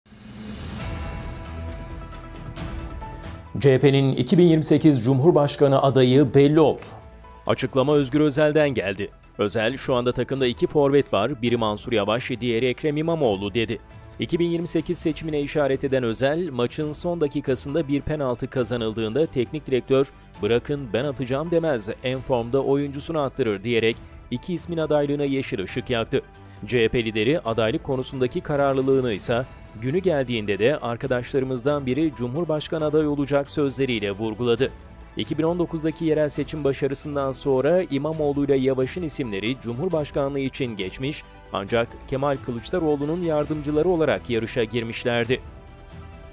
CHP 2028 Cumhurbaşkanı Adayını Açıkladı
CHP’nin 2028 Cumhurbaşkanı adayı belli oldu.
Açıklama CHP lideri Özgür Özel’den geldi.
Özel, “Şu anda takımda iki forvet var. Biri Mansur Yavaş, diğeri Ekrem İmamoğlu.” Dedi.
2028 seçimine işaret eden Özel, “Maçın son dakikasında bir penaltı kazanıldığında teknik direktör, ‘Bırakın ben atacağım’ demez. En formda oyuncusuna attırır.” Diyerek 2 ismin adaylığına yeşil ışık yaktı.
CHP lideri adaylık konusundaki kararlılığını ise “Günü geldiğinde de arkadaşlarımızdan biri cumhurbaşkanı adayı olacak.” Sözleriyle vurguladı.
2019’daki yerel seçim başarısından sonra da İmamoğlu ile Yavaş’ın isimleri cumhurbaşkanlığı için geçmiş ancak Kemal Kılıçdaroğlu’nun yardımcıları olarak yarışa girmişlerdi.

CHP 2028 CUMHURBAŞKANI ADAYINI AÇIKLADI